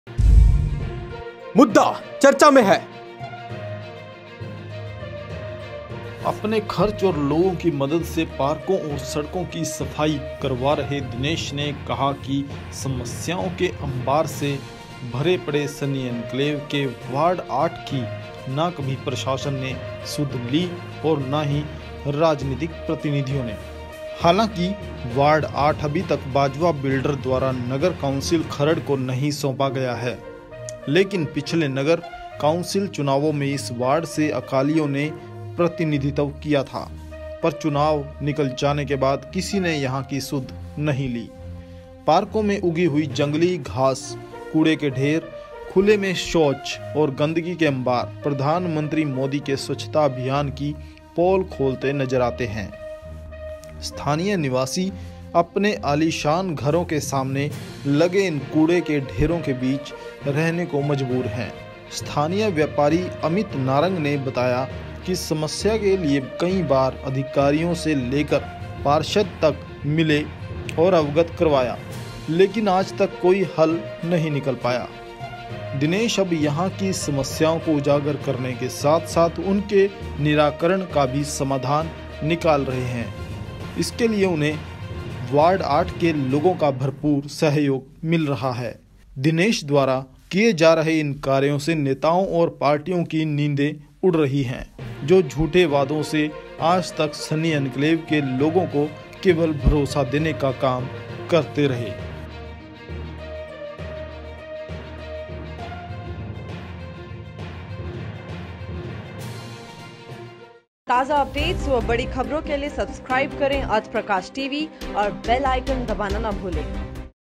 समाजसेवी दिनेश शर्मा ने सनी एंक्लेव में सफ़ाई कार्य शुरू करवाया
अपने खर्च और लोगों की मदद से पार्कों और सड़कों की सफ़ाई करवा रहे दिनेश ने कहा की
समस्याओं के अम्बार से भरे पड़े सनी एंक्लेव के वार्ड आठ की ना कभी प्रशासन ने सुध ली और नहीं राजनीतिक प्रतिनिधियों । हालाँकि वार्ड आठ अभी तक बाजवा बिल्डर द्वारा नगर काउन्सिल खरड़ को नहीं सौंपा गया है । लेकिन पिछले नगर काउन्सिल चुनावों में इस वार्ड से अकालियों ने प्रतिनिधित्व किया था। पर चुनाव निकल जाने के बाद किसी ने यहाँ कि सुध नहीं ली । पार्कों में उगी हुई जंगली घास, कूड़े के ढेर, खुले में शोच और गंदगी के अम्बार प्रधानमंत्री मोदी के स्वच्छता अभियान की पोल खोलते नज़र आते हैं । स्थानीय निवासी अपने आलीशान घरों के सामने लगे इन कूड़े के ढेरों के बीच रहने को मजबूर हैं। स्थानीय व्यापारी अमित नारंग ने बताया कि इस समस्या के लिए कई बार अधिकारियों से लेकर पार्षद तक मिले और अवगत करवाया लेकिन आज तक कोई हल नहीं निकल पाया । दिनेश अब यहाँ की समस्याओं को उजागर करने के साथ साथ उनके निराकरण का भी समाधान निकाल रहे हैं । इसके लिए उन्हें वार्ड आठ के लोगों का भरपूर सहयोग मिल रहा है । दिनेश द्वारा किए हाँ रहे इन कार्यों से नेताओं और पार्टियों की नींदें उड़ रही हैं जो झूठे वादों से आजतक सनी enclave के लोगों को केवल भरोसा देने का काम करते थे ।समाजसेवी दिनेश शर्मा  ने सनी एंक्लेव में सफ़ाई कार्य शुरू करवाया